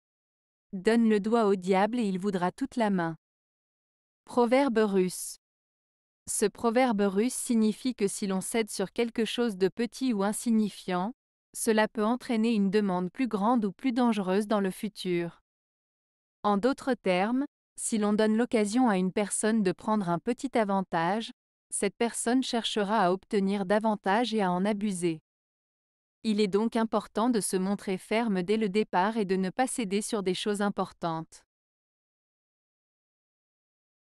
Donne le doigt au Diable et il voudra toute la main. - Proverbe Russe
Ce proverbe russe signifie que si l'on cède sur quelque chose de petit ou insignifiant, cela peut entraîner une demande plus grande ou plus dangereuse dans le futur. En d'autres termes, si l'on donne l'occasion à une personne de prendre un petit avantage, cette personne cherchera à obtenir davantage et à en abuser. Il est donc important de se montrer ferme dès le départ et de ne pas céder sur des choses importantes.